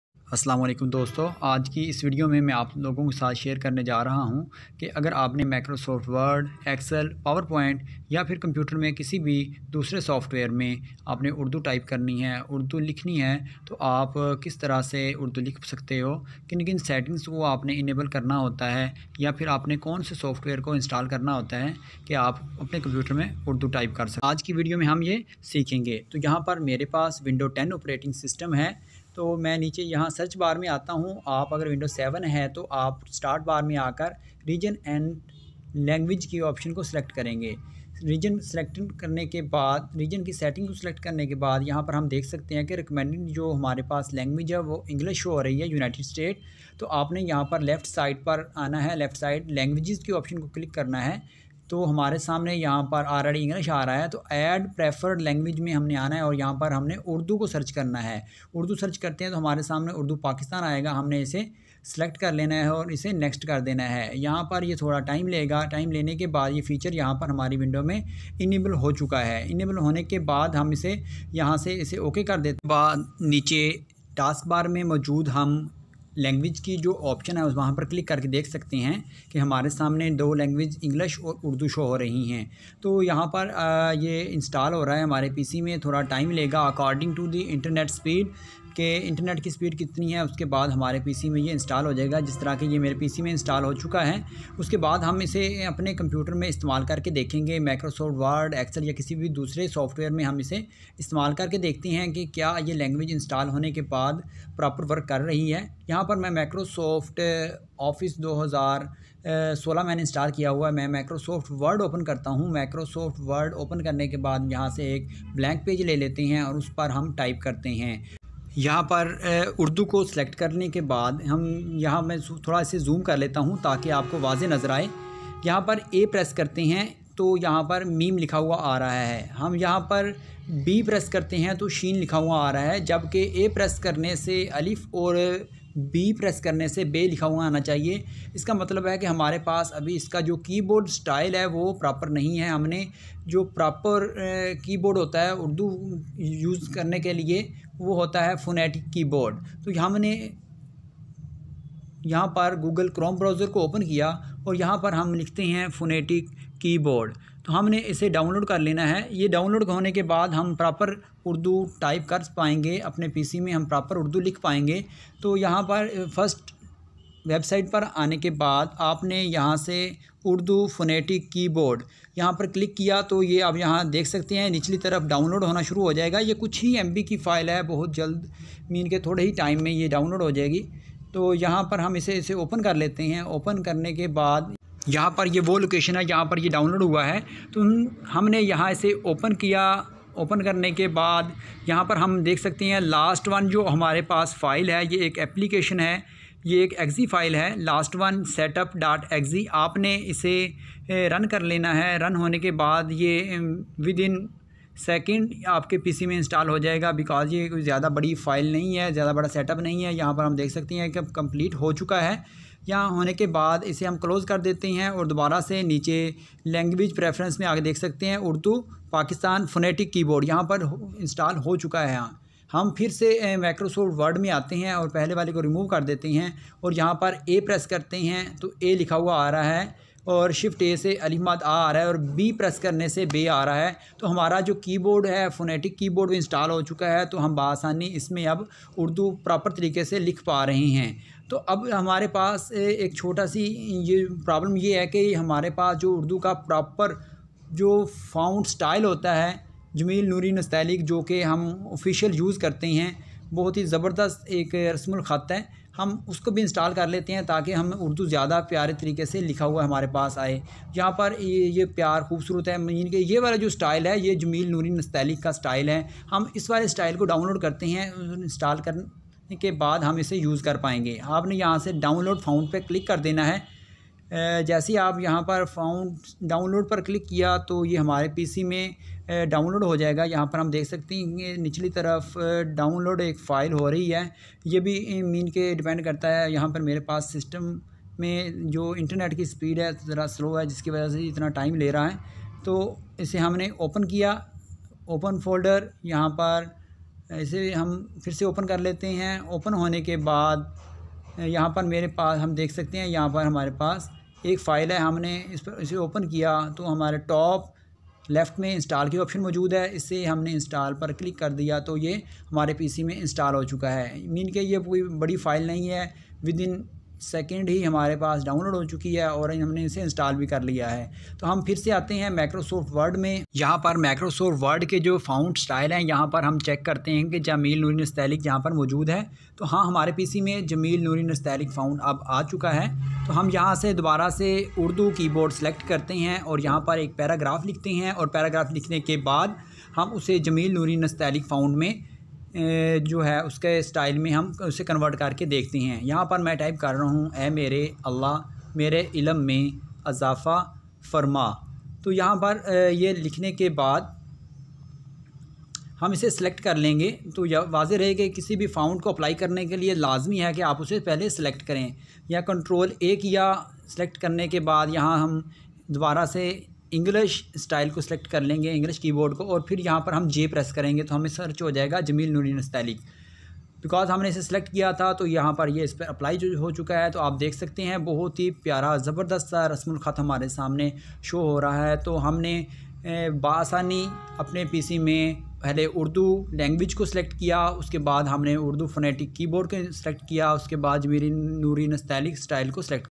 How to Write Urdu in Windows 10 | How to Write Urdu in MS Word, Excel, Power Point
In this video we will learn How to Write Urdu in Windows 10 | How to Write Urdu in MS Word, Excel, Power Point. by default there are English language selected in windows  operating system. if you would like to write or type Urdu in Window 10 then this video will relay helpful for you. even any software like Ms Office Ms word MS Excel, MS Power Point abode Photoshop, coral draw, abode illustrator, filmora and camtasia etc. there are some settings required to getting enabled to write Urdu in Windows 10 so complete watch this video.

Video Timestamp
0:00 Video Introduction
0:26 How to Enable Urdu Langue
1:42 Let's Check Urdu Langue
2:18 Install phonetic Keyboard
4:01 Install Jameel Noori Font
Check our Products and support us